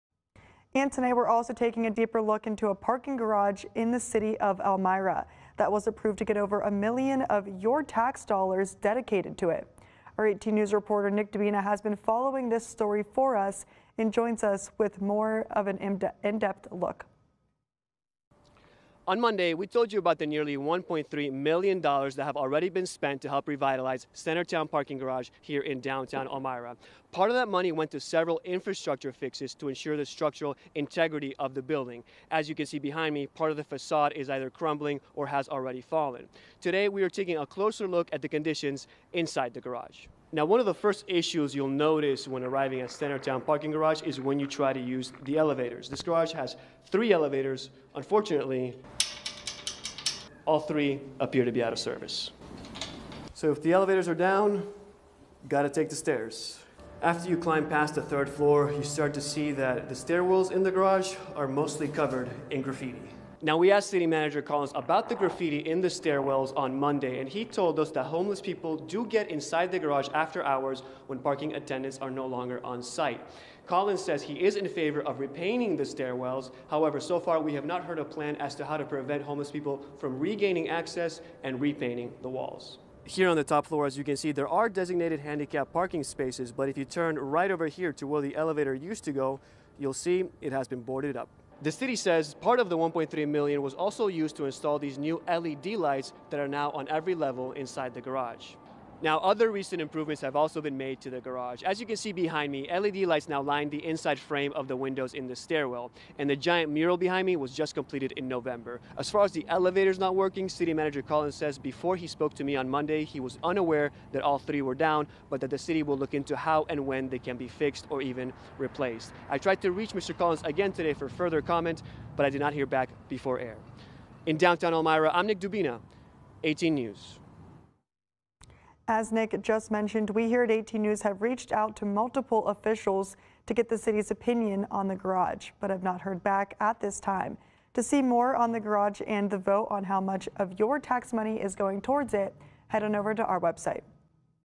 Closer look at problems inside Centertown Parking Garage in downtown Elmira
ELMIRA, N.Y. (WETM) – After the City of Elmira approved a nearly $1.3 million investment into the aging Centertown Garage on Gray Street, 18 News is taking a closer look at some of the issues facing the 45 year-old structure.